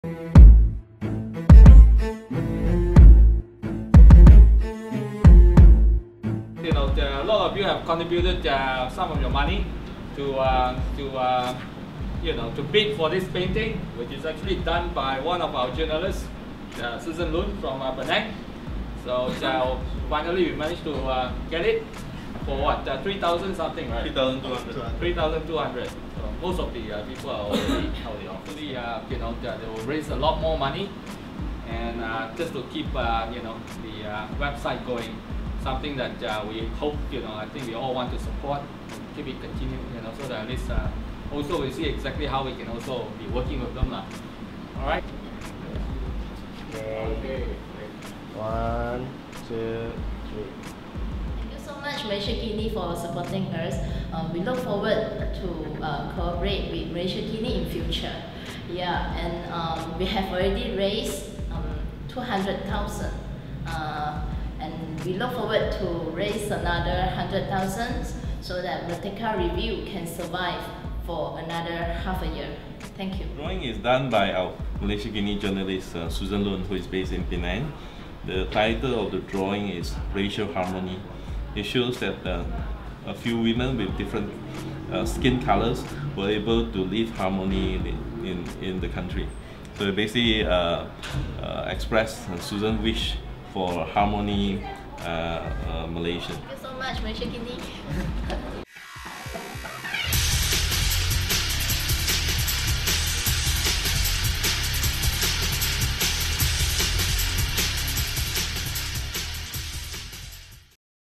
Malaysiakini to Merdeka Review's aid
Staff of Malaysiakini have risen to the aid of independent Chinese news portal Merdeka Review, which may face closure because its funders have abruptly backed out the venture.

They bid RM3,200 and won one of four paintings by Malaysiakini's Penang-based journalist Susan Loone, who had donated these for auction online as part of the 'Save Merdeka Review' campaign.